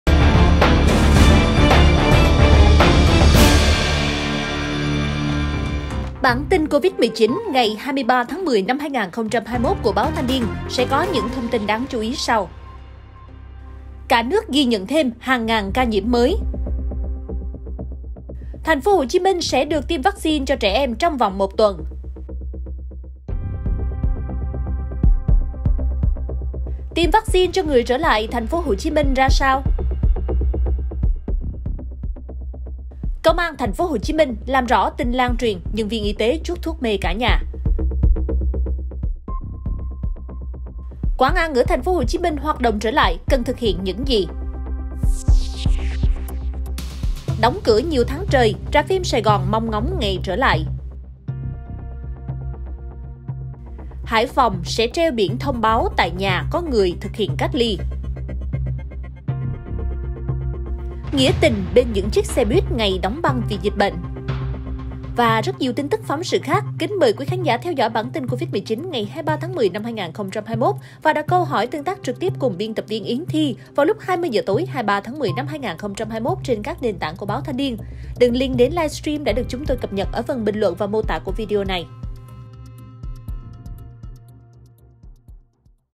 Đón xem bản tin Covid-19 ngày 23.10: TP.HCM sẽ tiêm vắc xin cho trẻ em trong vòng 1 tuần
Bản tin Covid-19 ngày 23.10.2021 của Báo Thanh Niên sẽ có những thông tin đáng chú ý sau:
👉 Cả nước ghi nhận thêm hàng ngàn ca nhiễm mới
👉 TP.HCM sẽ được tiêm vắc xin cho trẻ em trong vòng 1 tuần
👉 Tiêm vắc xin cho người trở lại TP.HCM ra sao?
👉 Công an TP.HCM làm rõ tin lan truyền 'nhân viên y tế chuốc thuốc mê cả nhà'
👉 Quán ăn ở TPHCM hoạt động trở lại cần thực hiện những gì?
👉 Đóng cửa nhiều tháng trời, rạp phim Sài Gòn mong ngóng ngày trở lại
👉 Hải Phòng sẽ treo biển thông báo tại nhà có người thực hiện cách ly
👉 Nghĩa tình bên những chiếc xe buýt ngày “đóng băng” vì dịch bệnh

Và rất nhiều tin tức, phóng sự khác.
Kính mời quý khán giả theo dõi bản tin Covid-19 ngày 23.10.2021 và đặt câu hỏi tương tác trực tiếp cùng biên tập viên Yến Thi vào lúc 20 giờ tối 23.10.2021 trên các nền tảng của Báo Thanh Niên.
Đường link đến livestream đã được chúng tôi cập nhật ở phần bình luận và mô tả của video này.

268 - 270 Nguyễn Đình Chiểu, Phường Võ Thị Sáu, Q.3, TP HCM